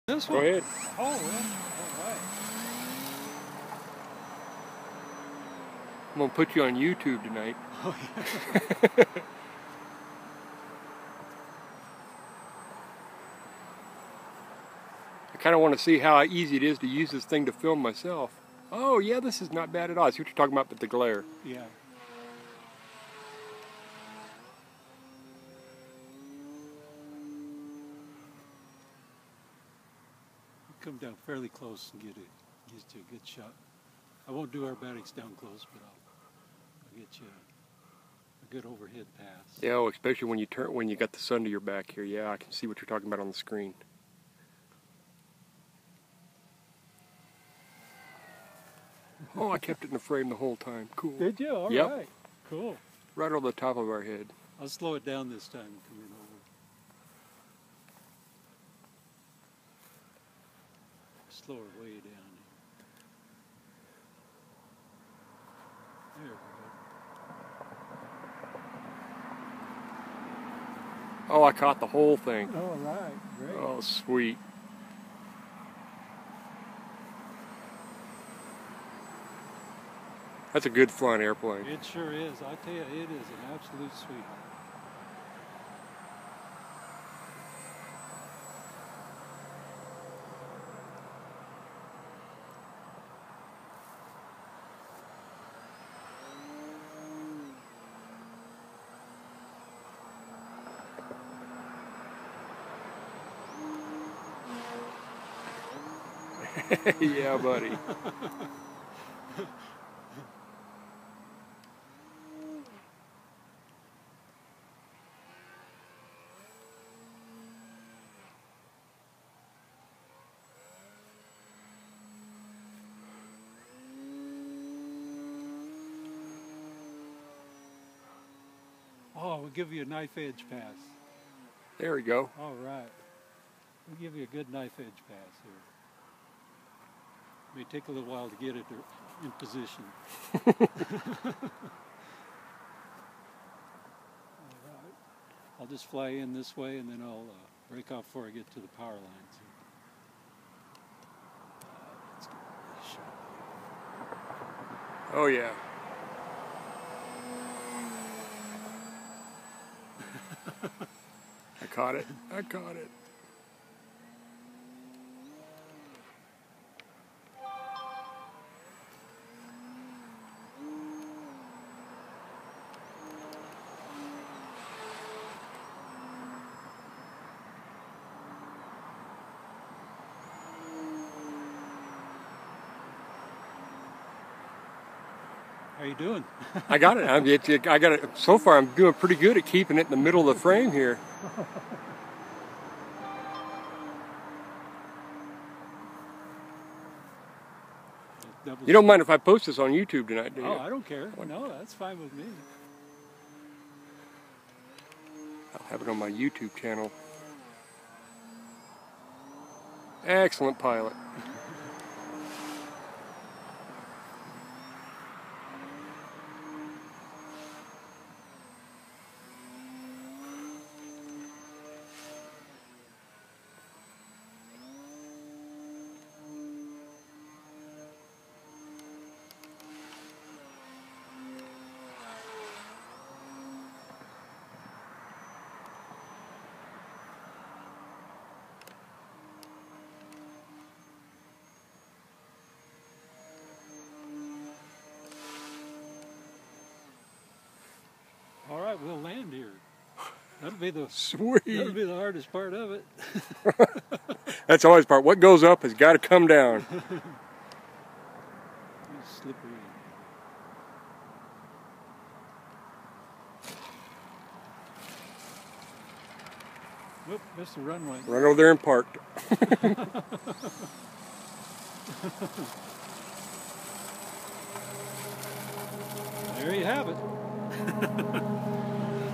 Flight of my filming buddies Timber
Flown by Dave Pogue from Mesa Arizona. Outstanding RC Pilot